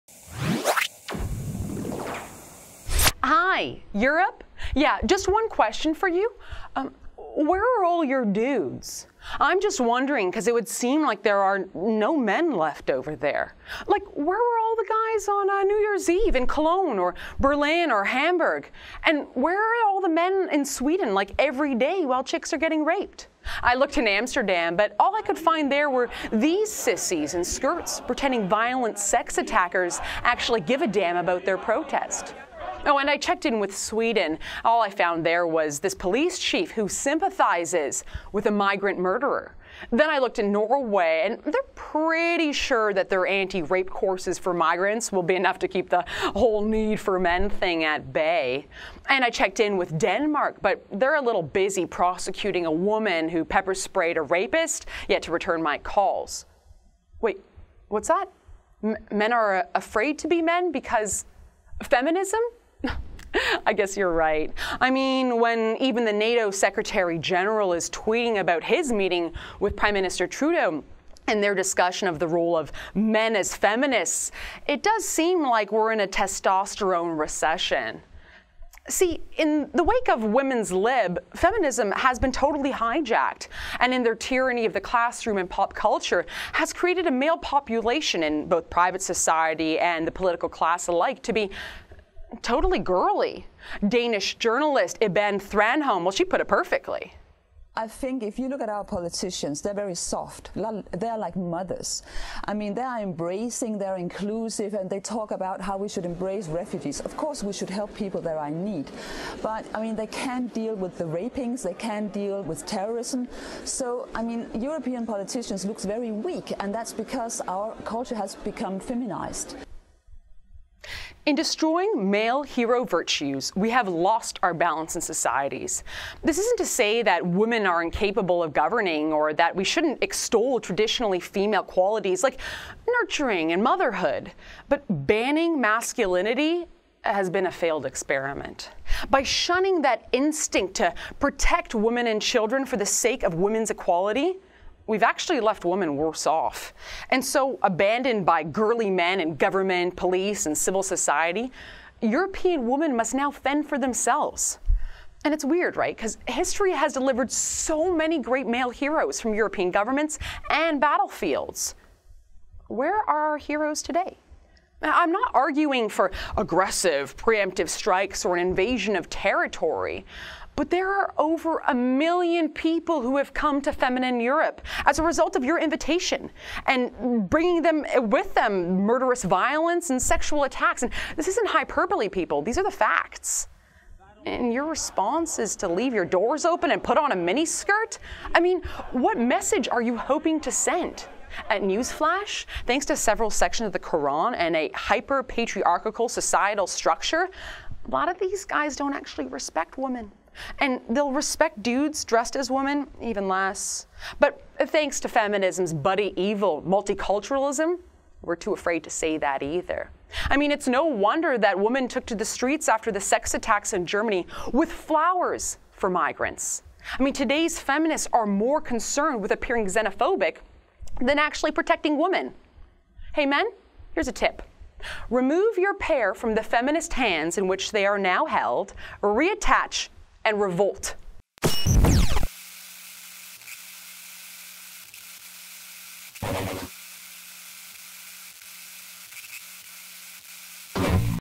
Where are Europe's men? Faith Goldy on the 'testosterone recession'
While women in Germany, Denmark, Sweden and elsewhere are being groped and raped by Muslim migrants, many are asking, "Where are their men?" As Faith Goldy of TheRebel.media notes, the ones that aren't wearing miniskirts at protest rallies (!) are apparently doing nothing at all. MORE